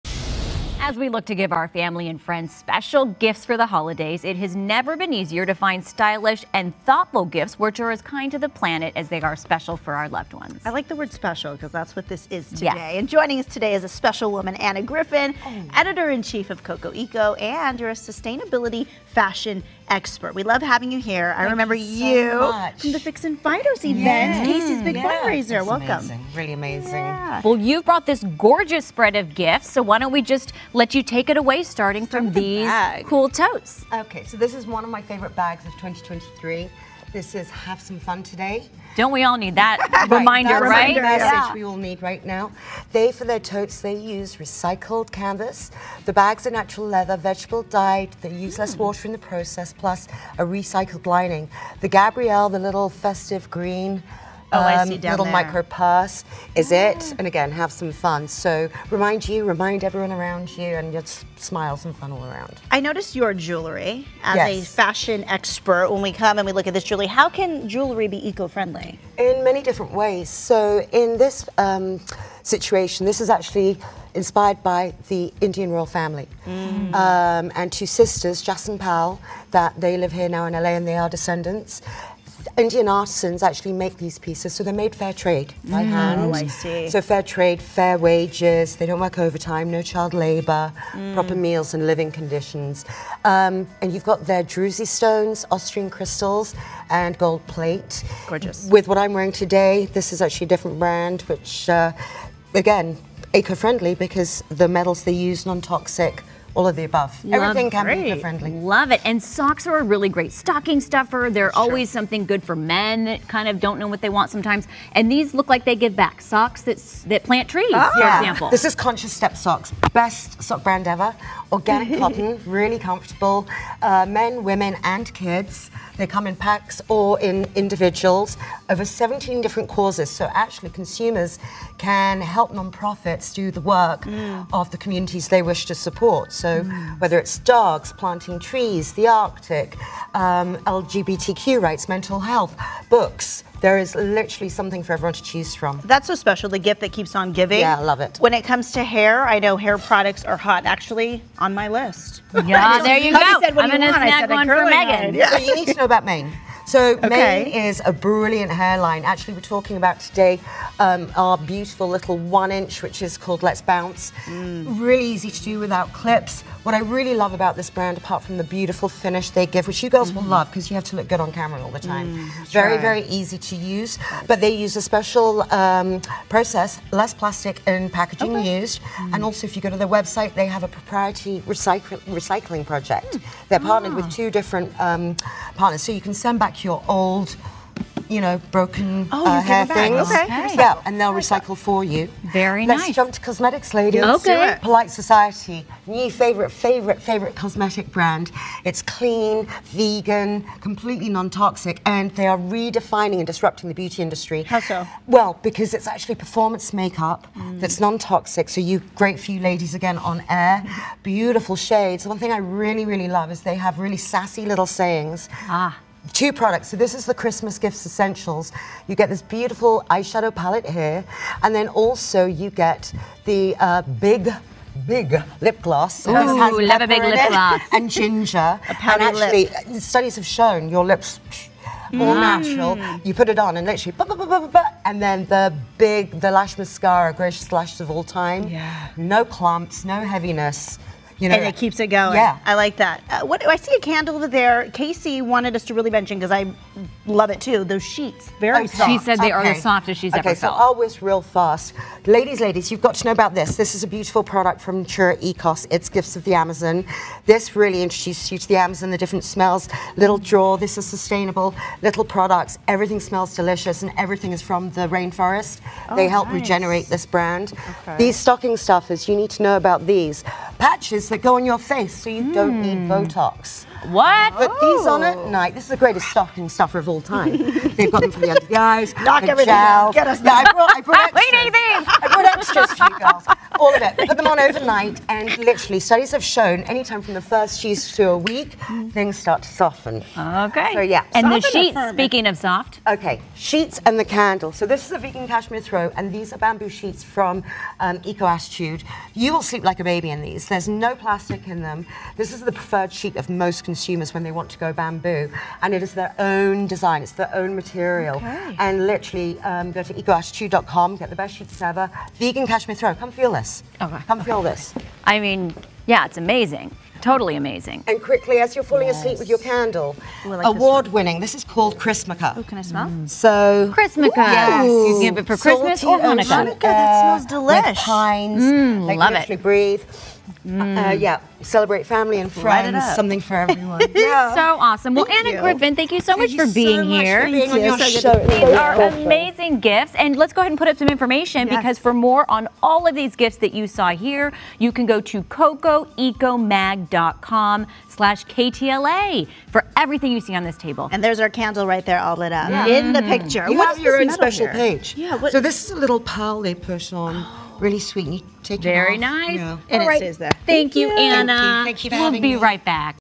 Eco-friendly holiday gift ideas
Anna Griffin, editor-in-chief of CoCo Eco, joined KTLA Weekend Morning News to share some sustainable holiday gift ideas. Dec. 10, 2023.